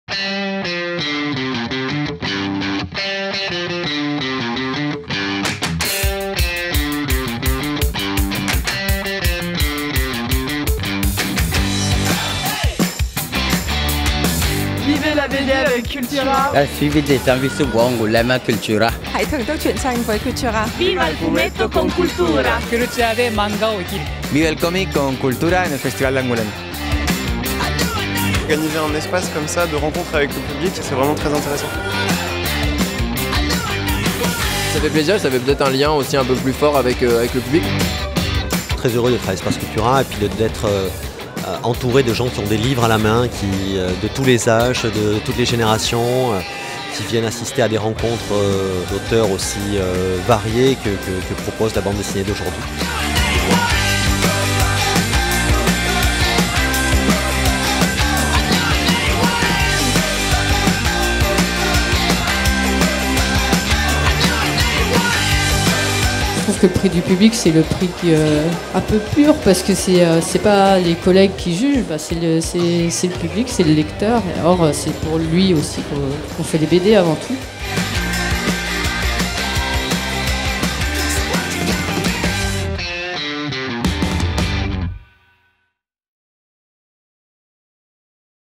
Cultura au Festival de la BD d'Angoulême 2018
La 45ème édition du Festival International de la Bande Dessinée d’Angoulême, s’est achevée. Pour la 6ème année, Cultura est partenaire de cet événement incontournable pour tous les amoureux de la bande dessinée. Durant 4 jours, l'espace Cultura s'est rythmé des rencontres d'auteurs de la sélection du Prix du Public Cultura, d'ateliers créatifs, des découvertes de jeux et bien d'autres moments de partage autour de la BD. Retour en vidéo sur les temps forts de Cultura au Festival d’Angoulême…